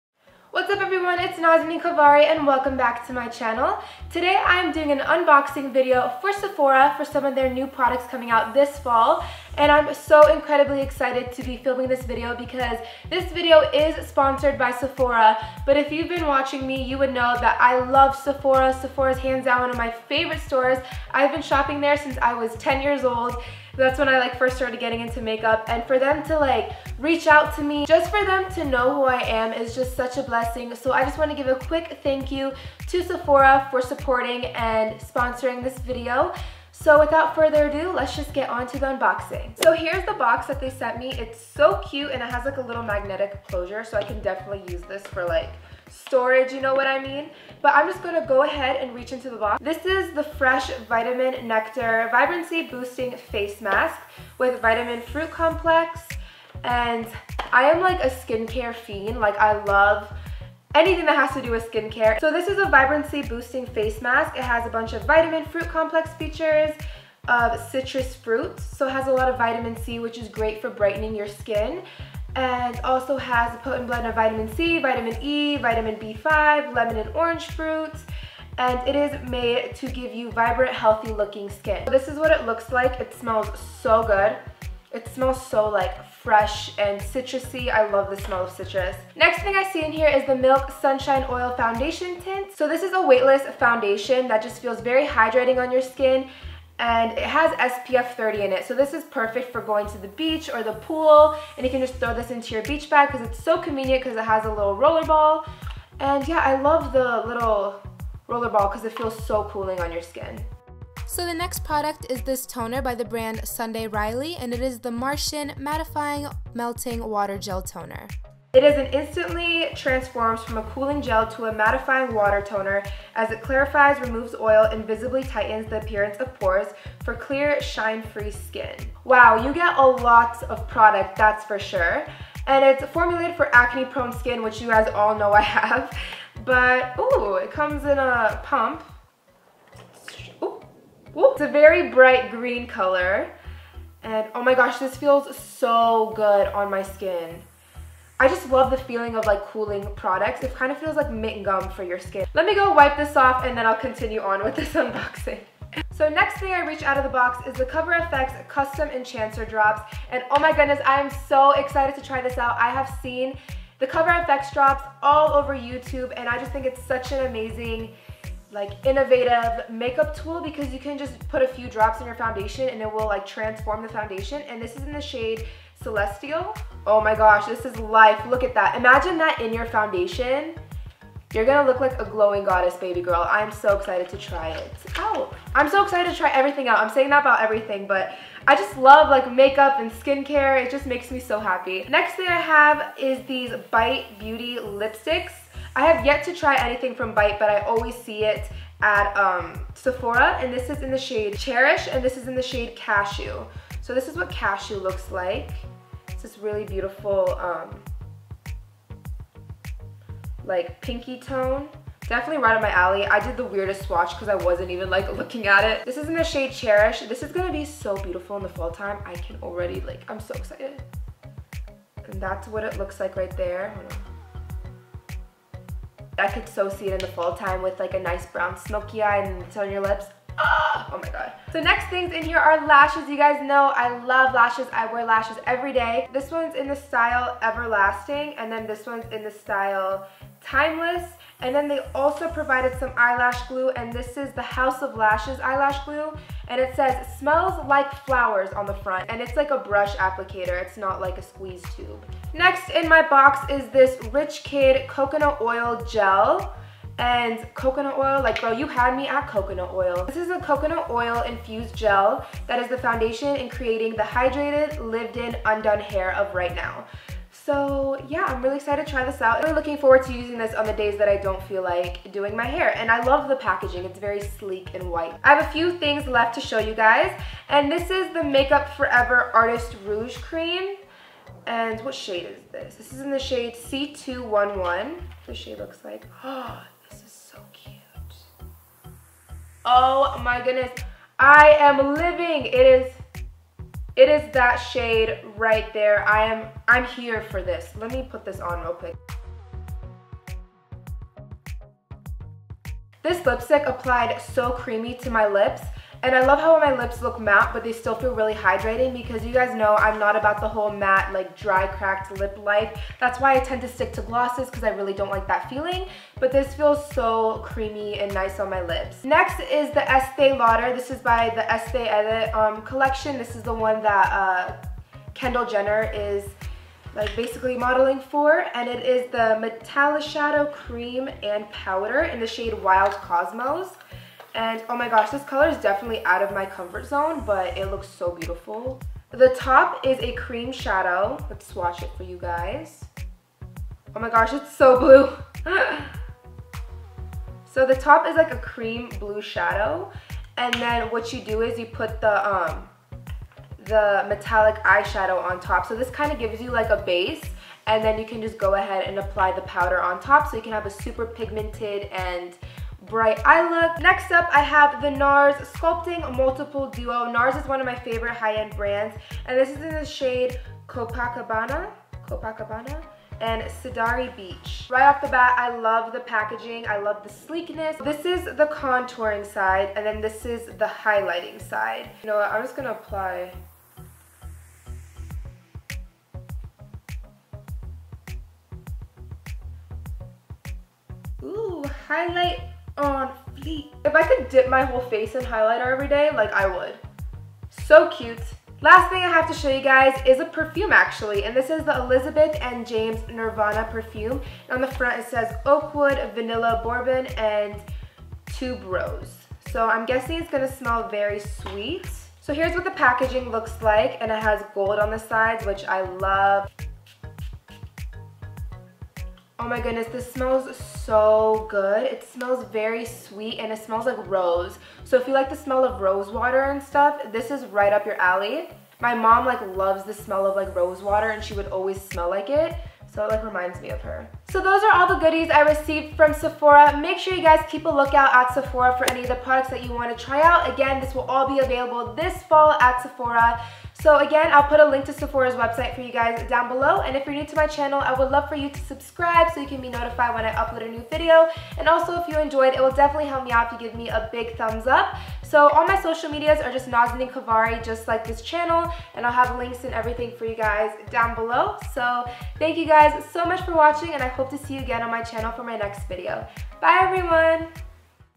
WHAT'S NEW AT SEPHORA |UNBOXING♡
Watch in HD♡  THANK YOU GUYS SOOOO MUCH FOR 400K!! I love you guys and am so thankful for all your support! Hair care routine will be up this week♡

My sister and I's account @KavariBeauty

Nazanin Kavari

FTC- This video is sponsered by Sephora. All opinions are my own and are 100% honest